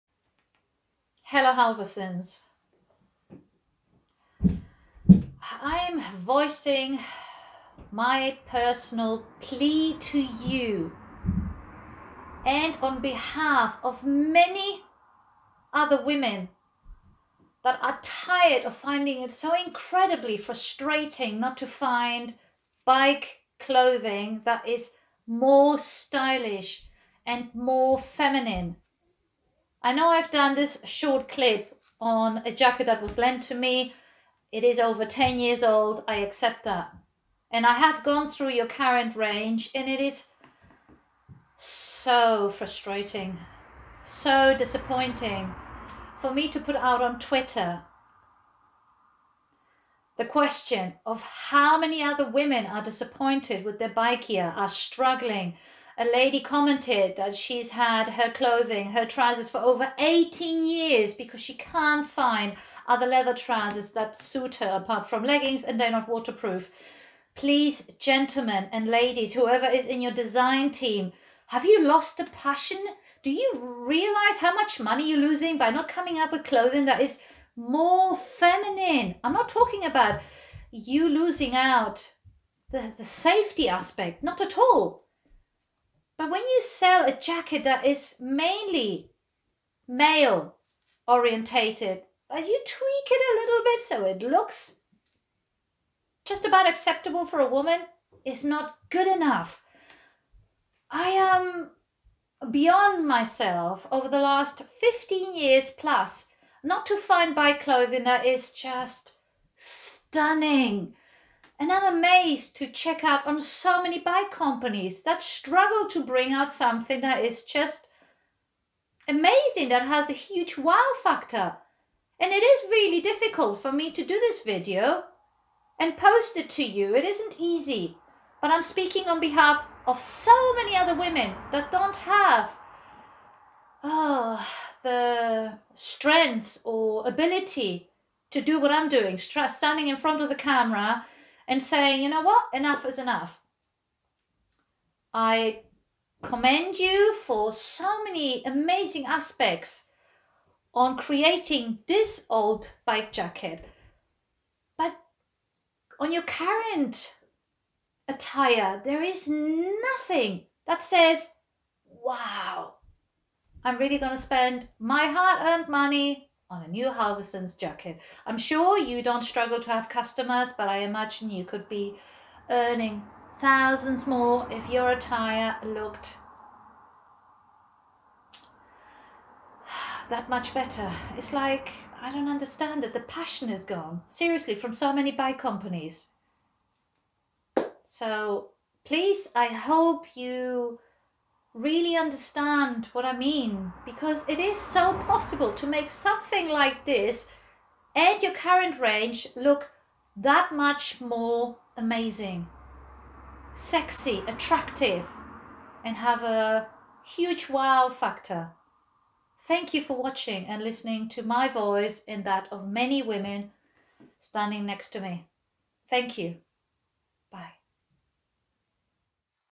Hello Halvarssons...
This video is speaking on behalf of many disappointed female bikers, that are TIRED and UNABLE to find motorbike clothing that represents our Femeninity with Style!